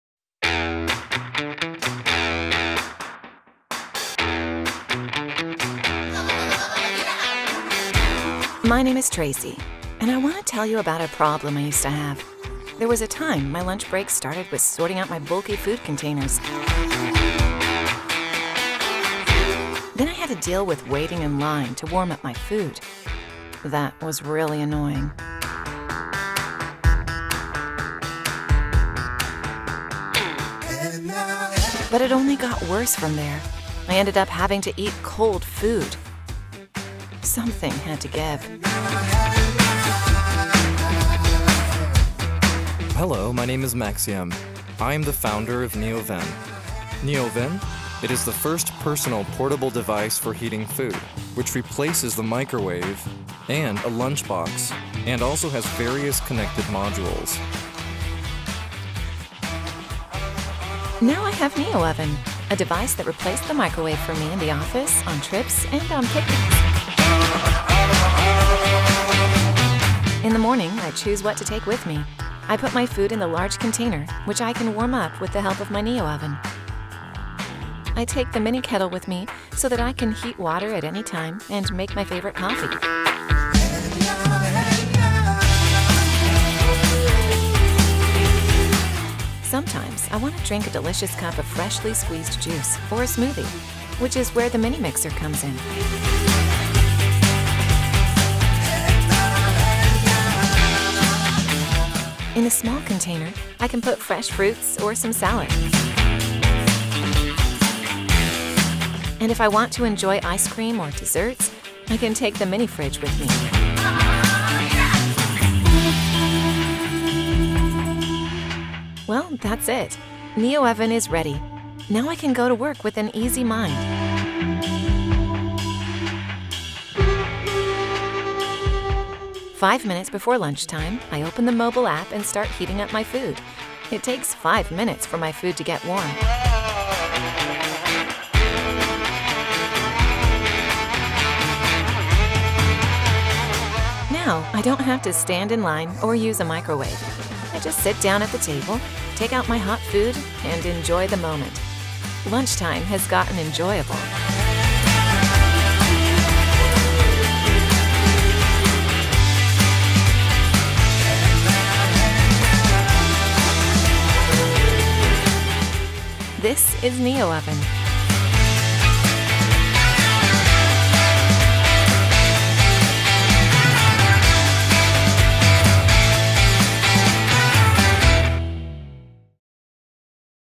Neoven - commercial video
Neoven - world's first portable food heating device with smart plug-ins - kettle,mixer and fridge.